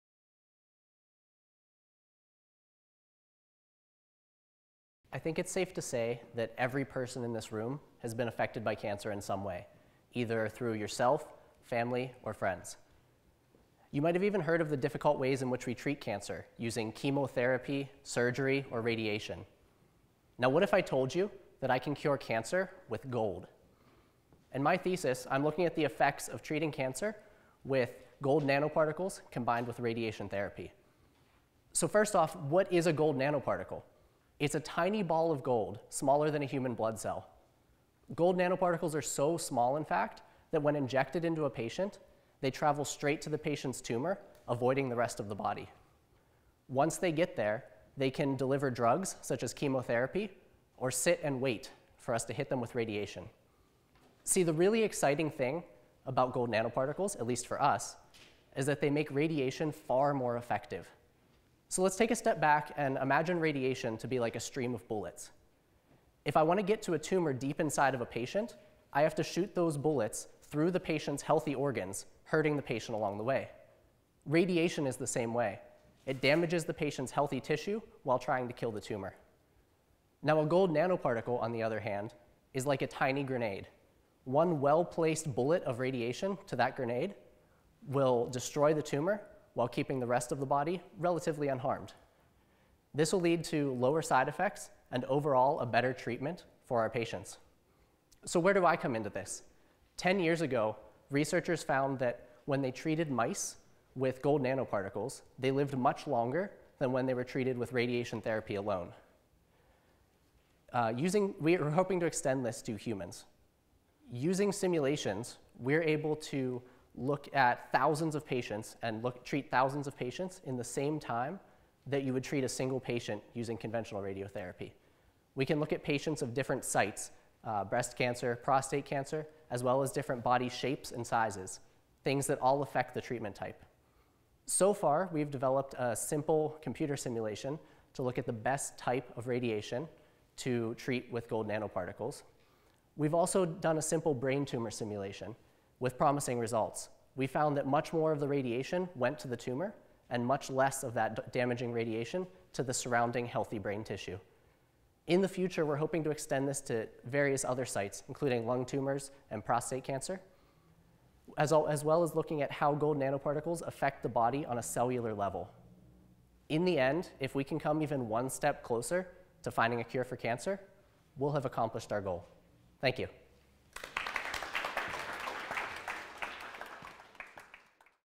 UCalgary Three Minute Thesis (3MT) 2015 - Brandon Koger
FINALIST
Brandon Koger, Physics and Astronomy
Supervisor: Dr. Charles Kirkby
Gold nanoparticle enhanced radiation therapy: A simulation study.

Visit ucalgary.ca/mygradskills for more information.